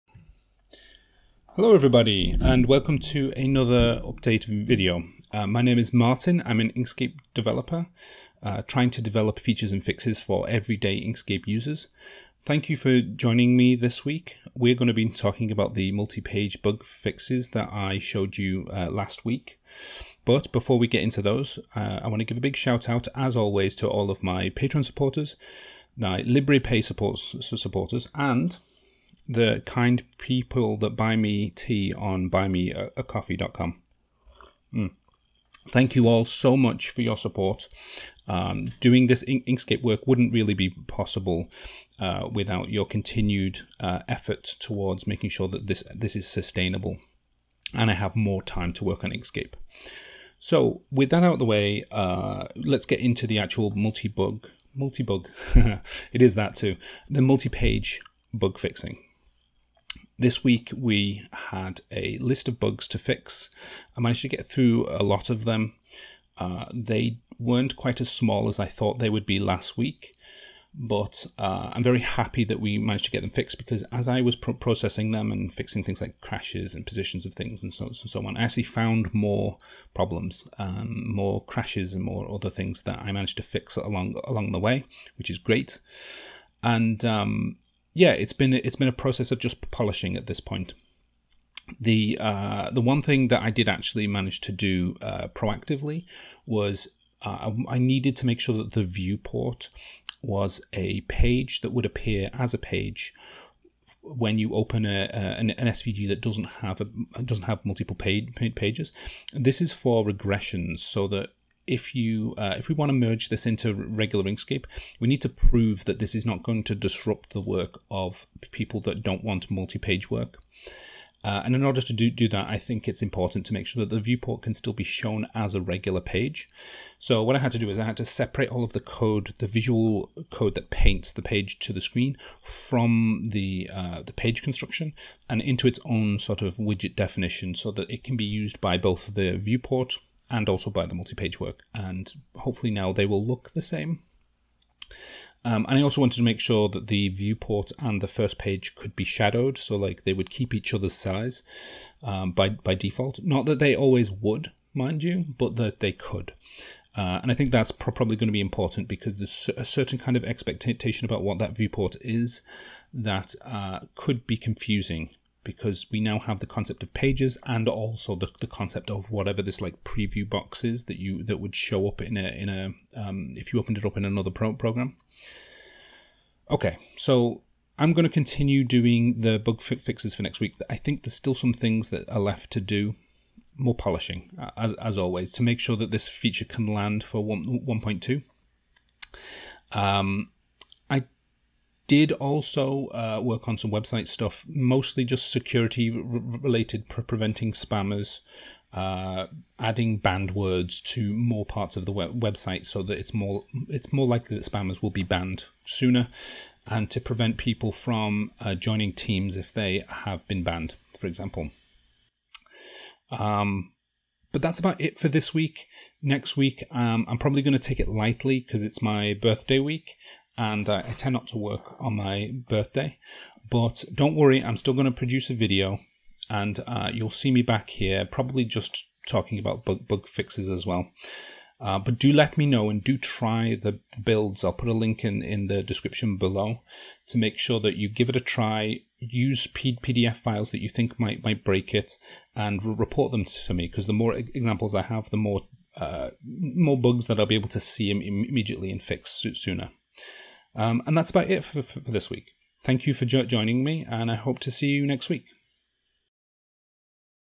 Are multi-pages buggy? - Inkscape Developer Update 23rd Oct 2021
Welcome everyone, this week is a polishing and bug fixing week with a viewport regression fixes which is mostly about making sure we can get this thing merged without too many problems.

Sorry if this sounded rushed, this was the fourth take, the camera would just NOT save the damn video this morning. Oh well!